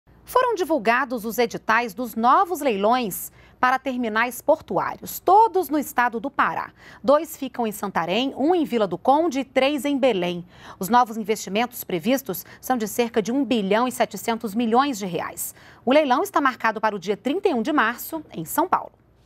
Terminais portuários do estado do Pará vão a leilão em São Paulo
NBR NOTÍCIAS - 25.01.16: Editais para novos leilões de terminais portuários no estado do Pará foram divulgados nesta segunda-feira (25).  Dois ficam em Santarém, um em Vila do Conde e três em Belém. Os investimentos previstos são de aproximadamente RS 1,7 bilhão. O leilão será realizado em São Paulo, no dia 1º de março.